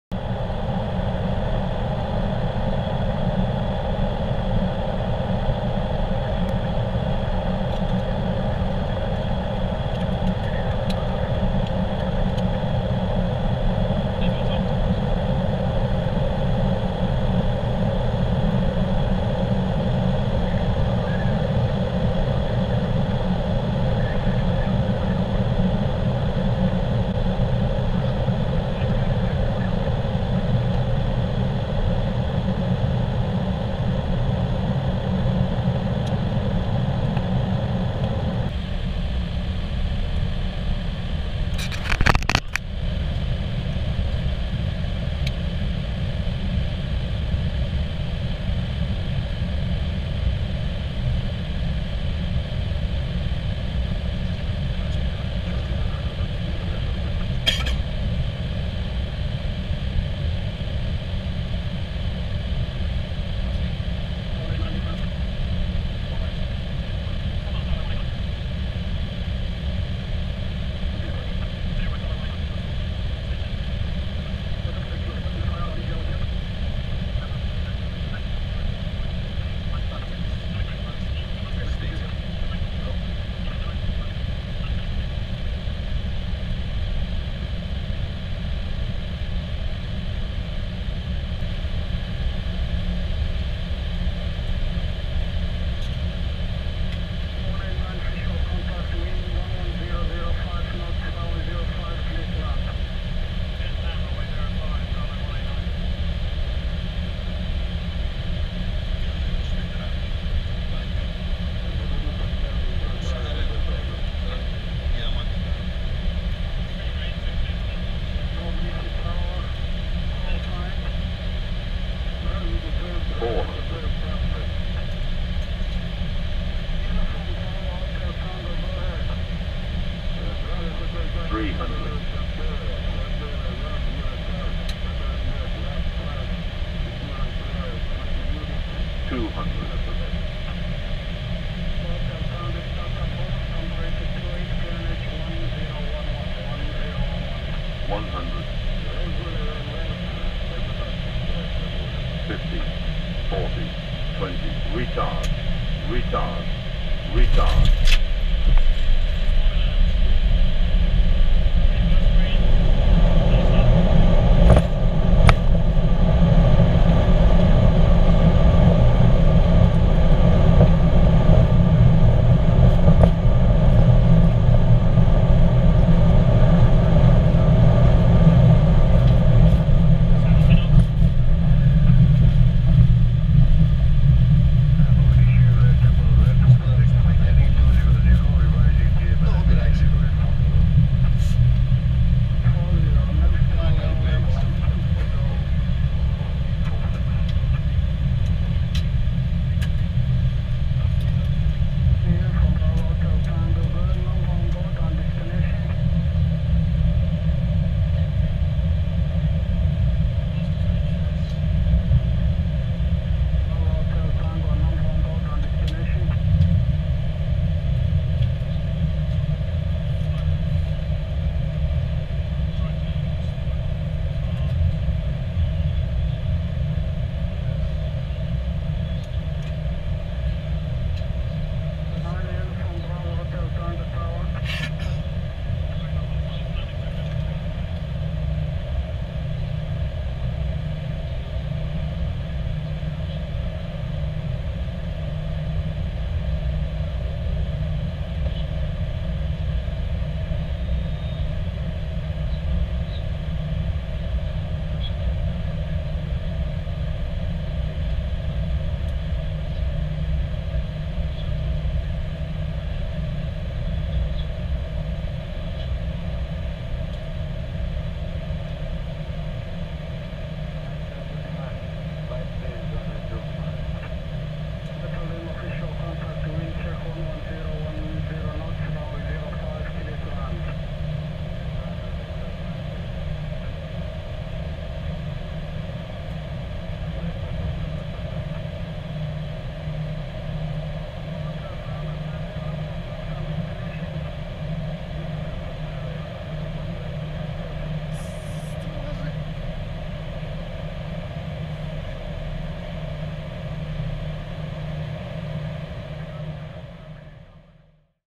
Mogadishu HCMM Cockpit view side windows
SOMALIA, Mogadishu MGQ / HCMM
A321 Cockpit view landing on rwy 05
Side windows view